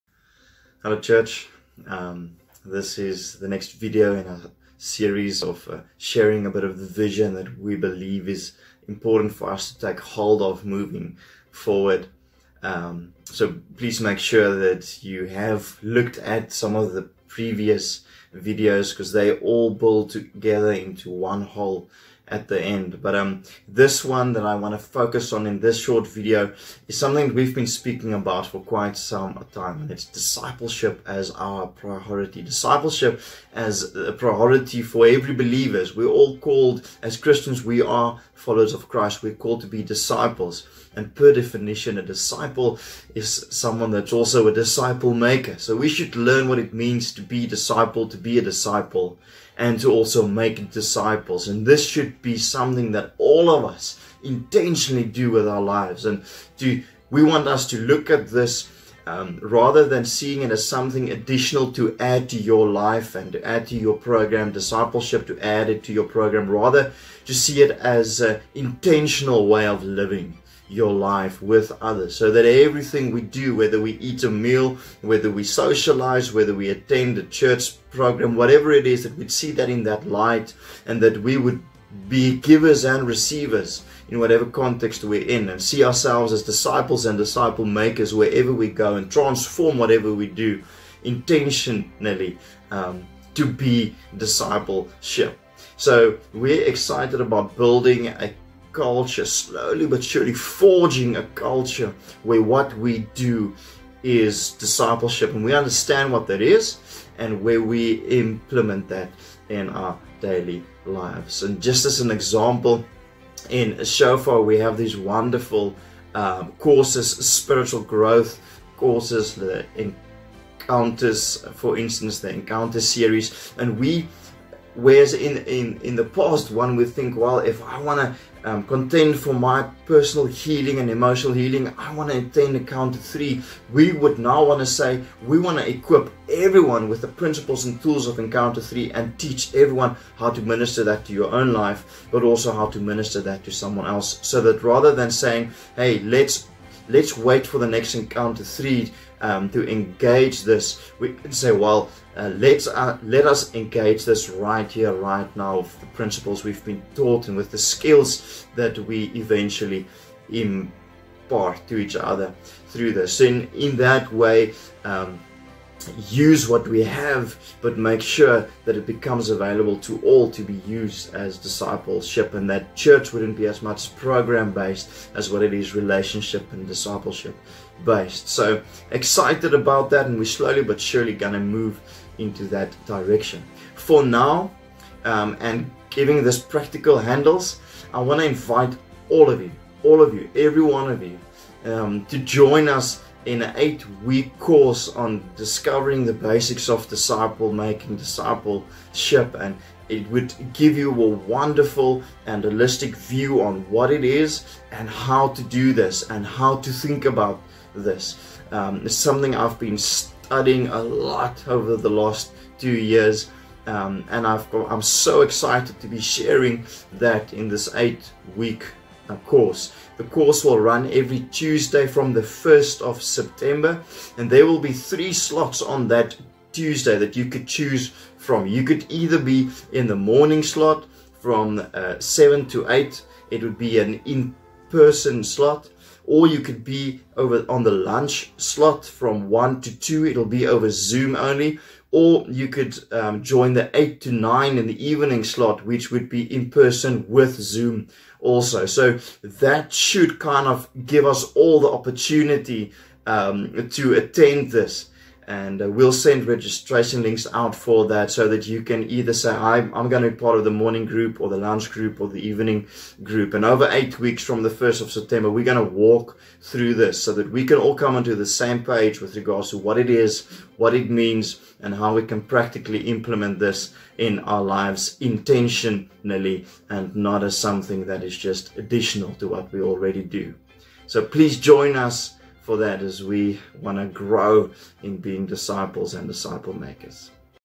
Unpacking Vision #2
DISCIPLESHIP AS OUR PRIORITY. Unpacking the next exciting component of what we believe God is leading us in and to take hold of for the next season.